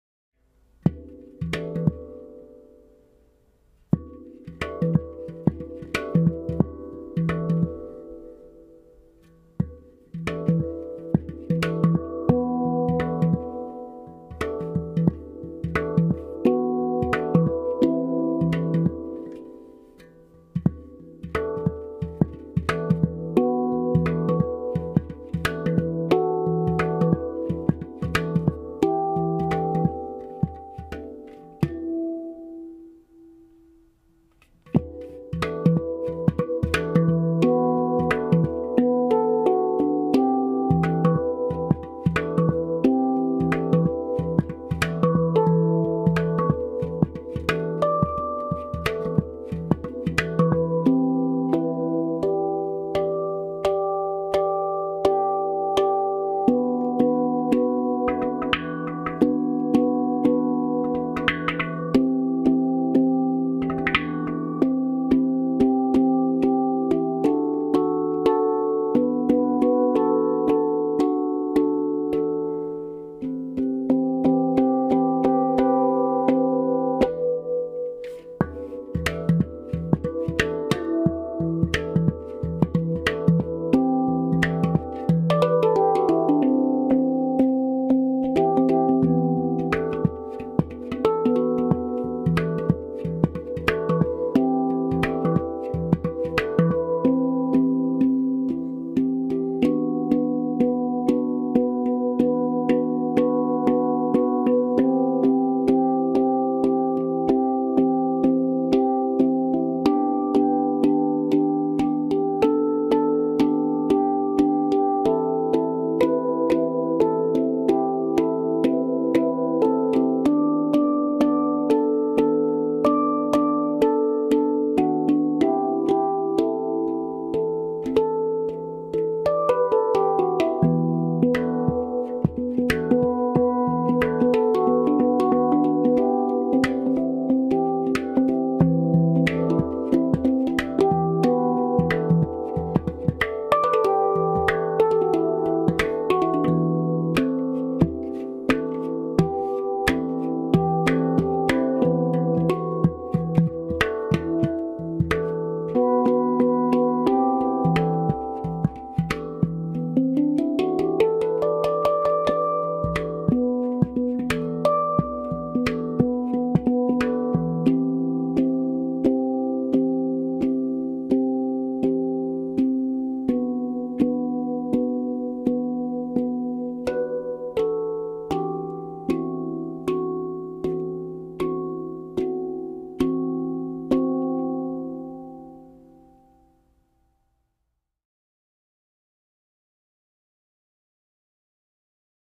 Tacta Handpans - E Kurd 10
Notes:
E B C D E F# G A B D
Recorded at Peter Levitov's studio in Gainesville, FL, June 2023
No effects added to sound recording